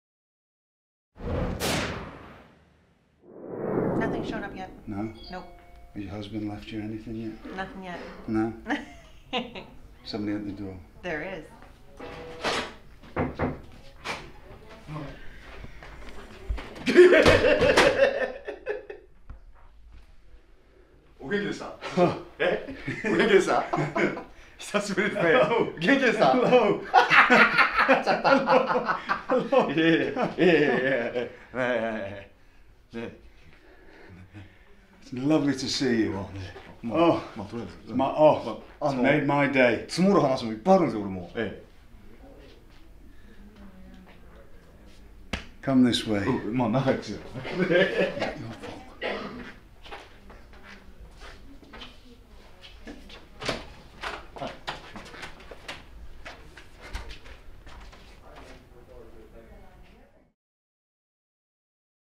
Tajiri and William Regal reunite: June 29, 2016
Longtime friends and former World Tag Team Champions Tajiri and William Regal reunite as The Japanese Buzzsaw prepares for the Cruiserweight Classic.